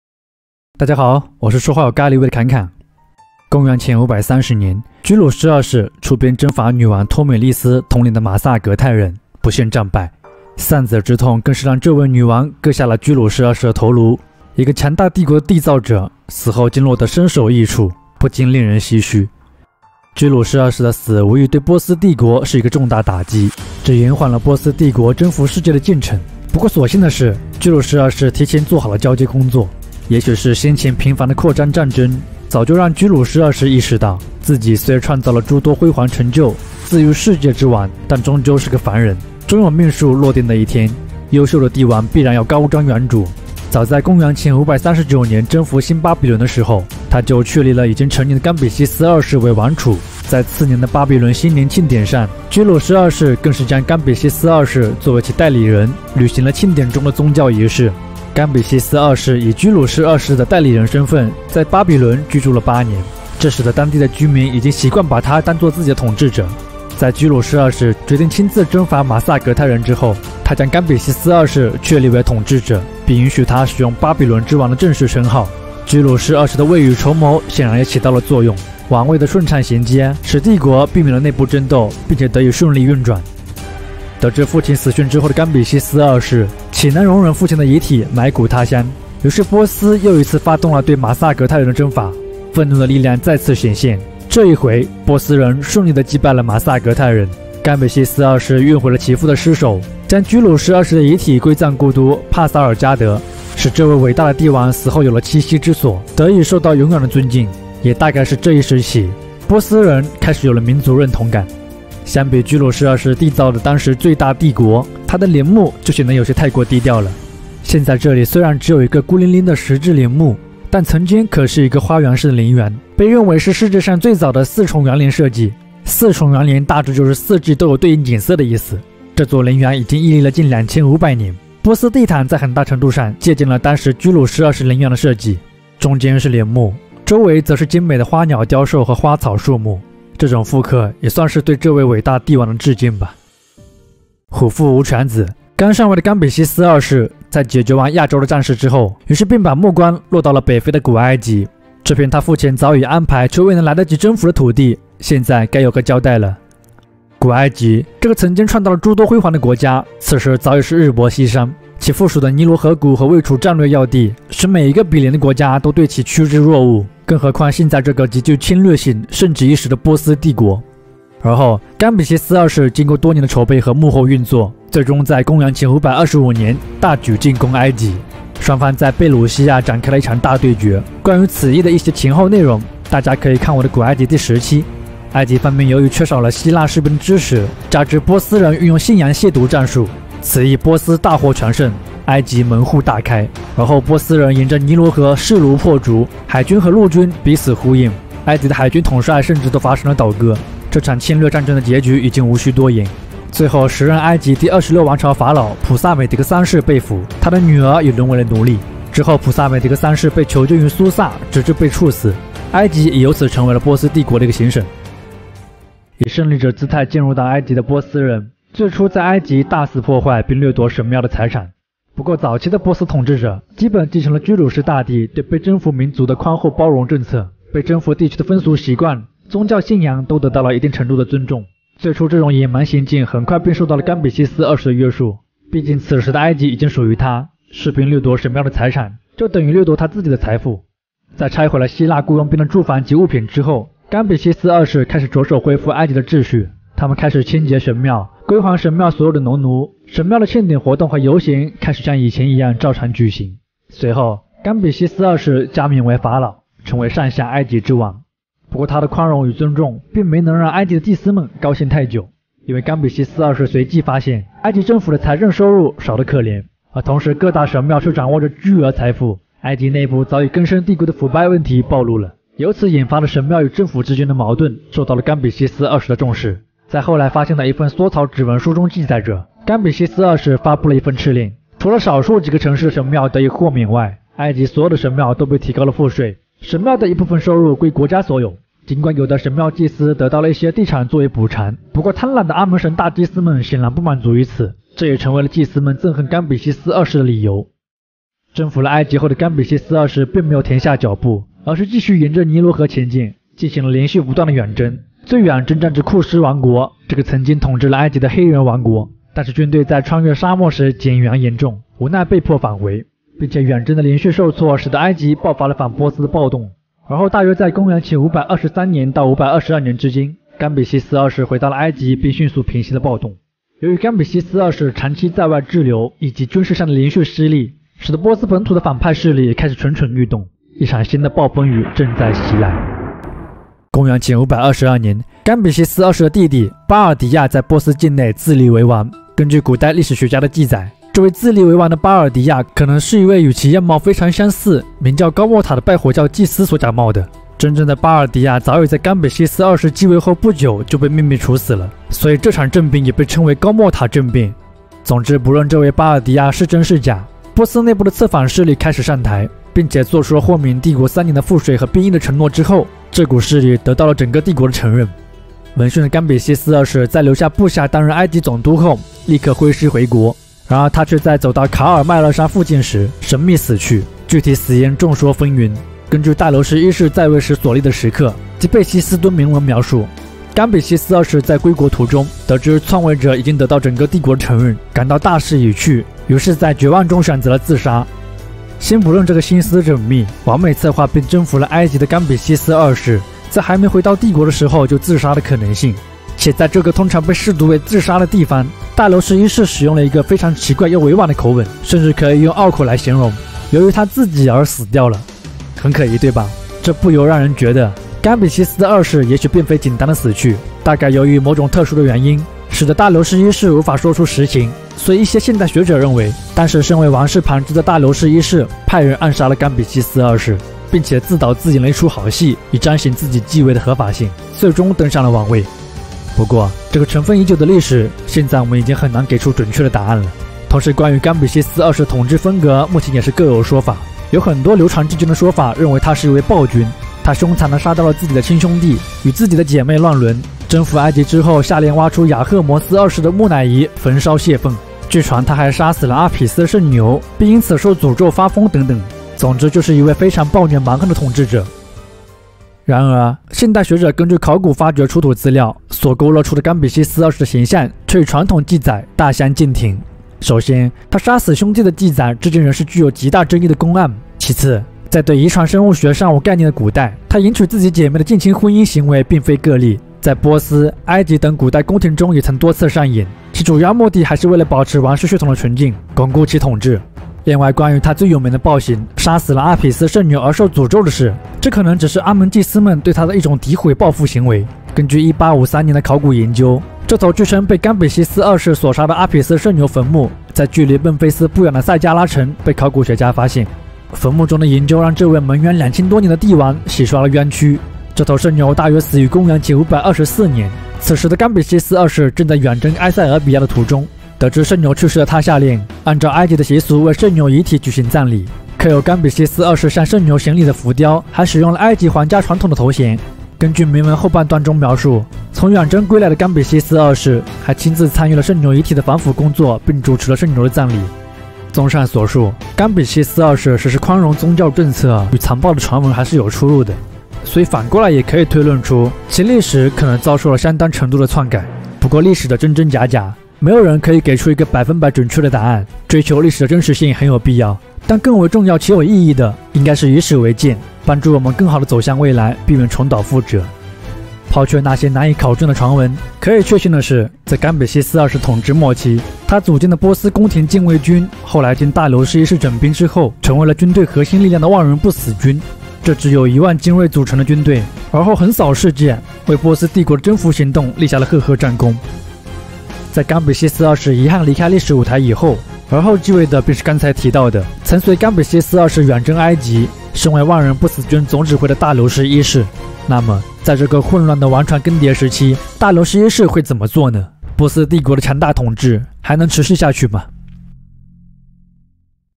【伊朗简史第三期】暴君？贤王？波斯帝国第二位皇帝，冈比西斯二世
冈比西斯二世
这位征伐了马萨格泰人，带回其父尸首，而后又征服了古埃及的统治者，最后为何神秘死亡？
另外，他究竟是如传闻中，是一位非常残暴的君主，还是一位贤王？
今天，就来侃侃这位，波斯帝国第二任皇帝吧！